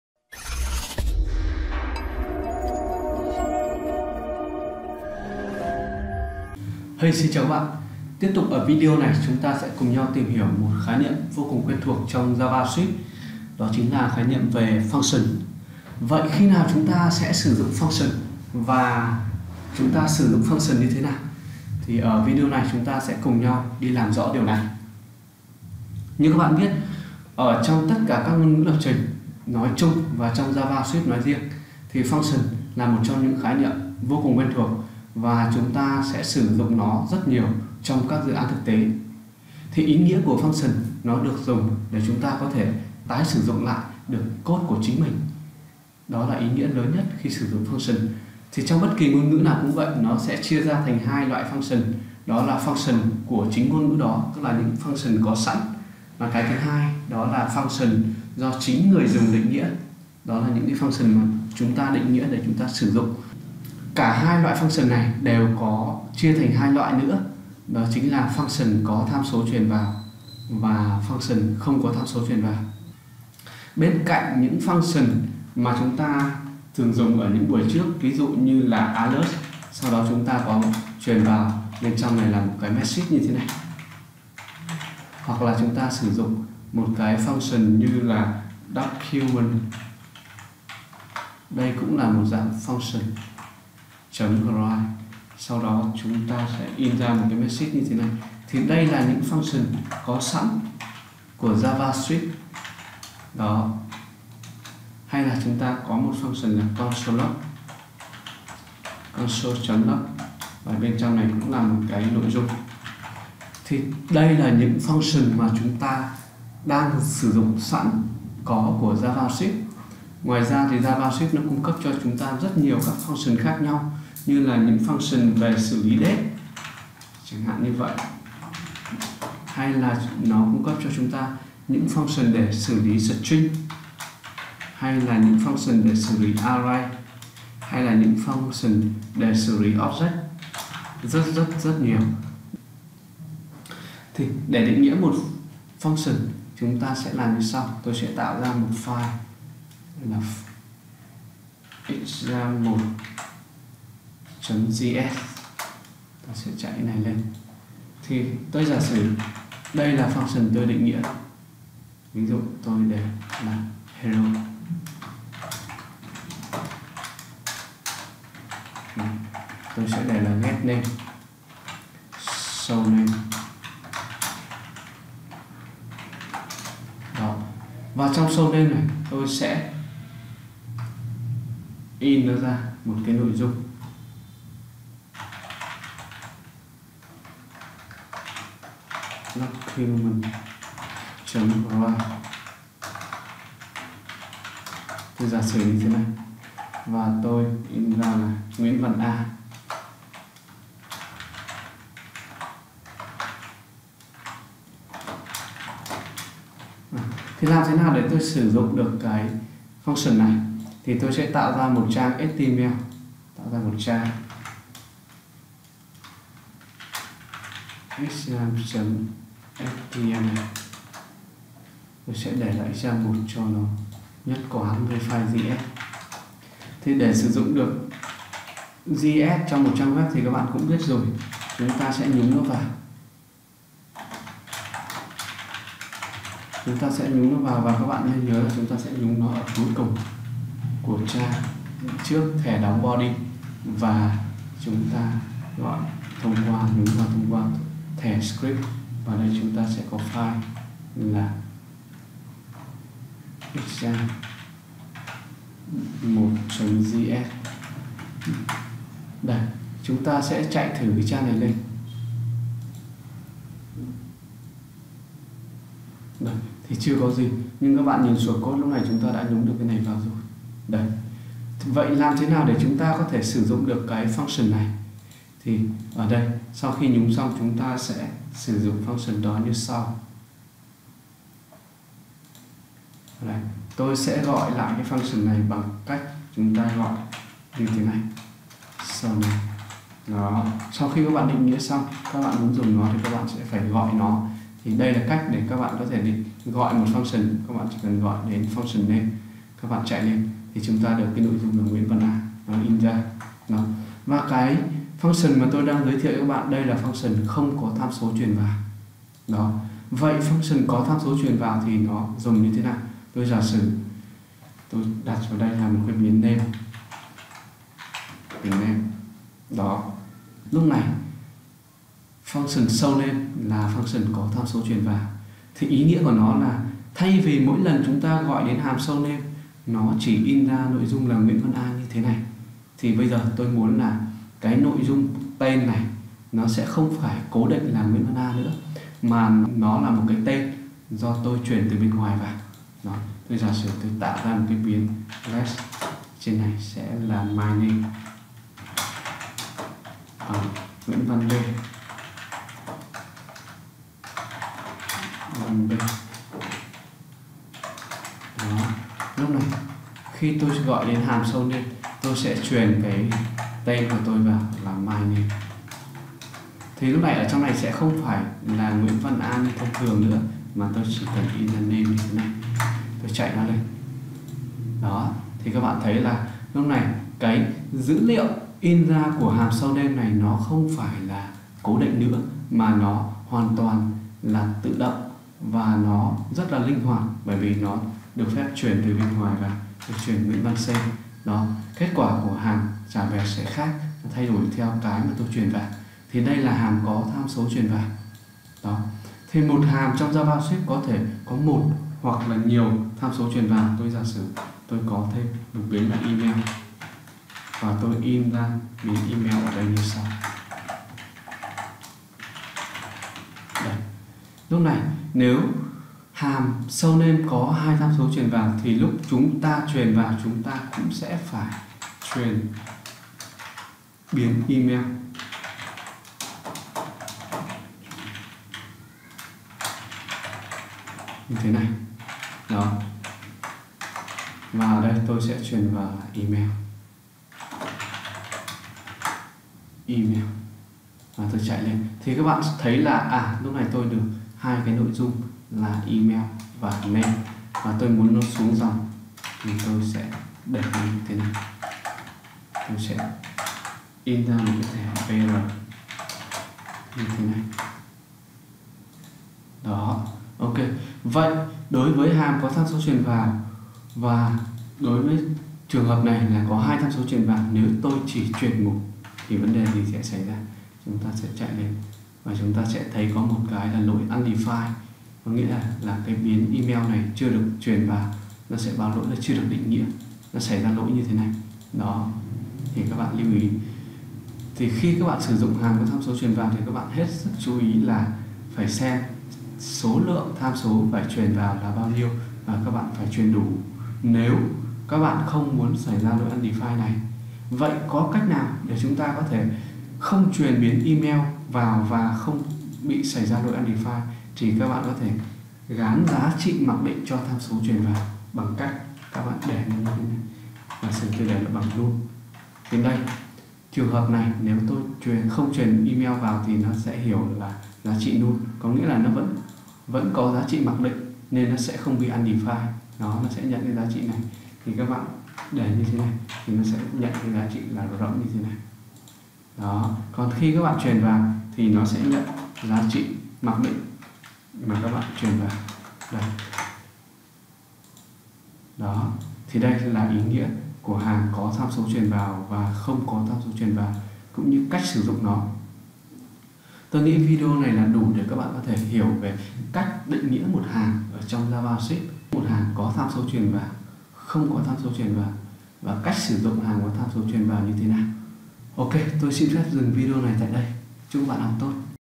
Function trong Javascript
Hàm trong Javascript - Function trong Javascript
Cách định nghĩa và sử dụng Function trong Javascript
Function có tham số truyền vào và không có tham số truyền vào.

Danh sách video khóa học lập trình Laravel tại SUNTECH